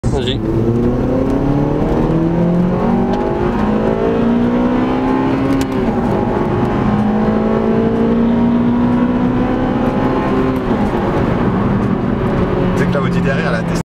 VTEC dans tout les sens.
Ceci a été réalisé sur une autoroute en Allemagne.